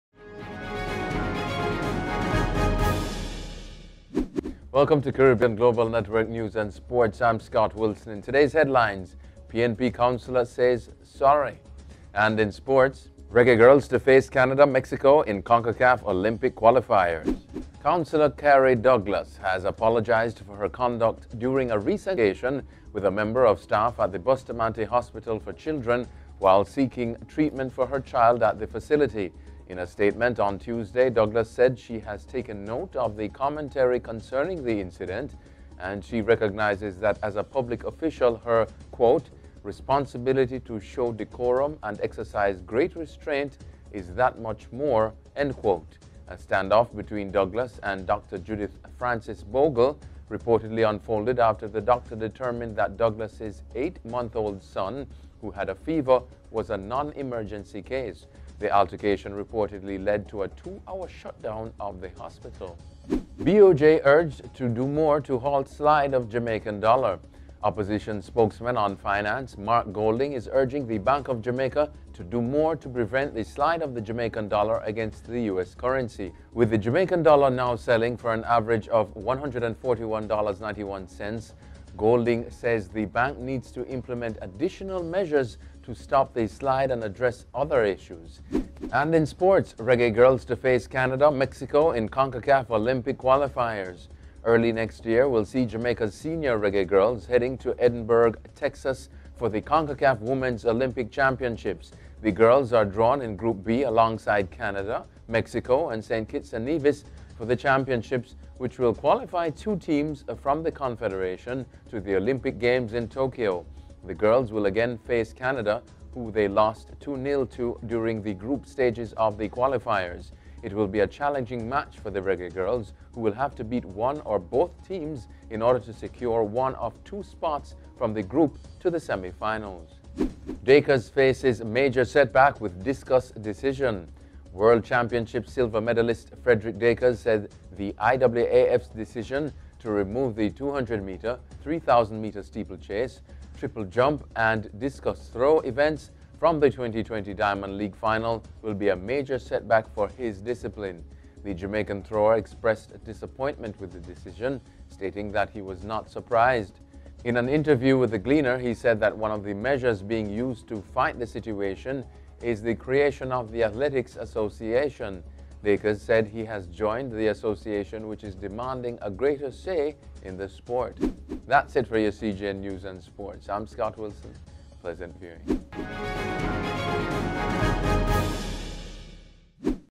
PNP Councillor says sorry - CGN News & Sports Ep 206
PNP Councillor says sorry. BOJ urged to do more to halt slide of Jamaican dollar. Reggae Girlz to face Canada, Mexico in Concacaf Olympic qualifiers.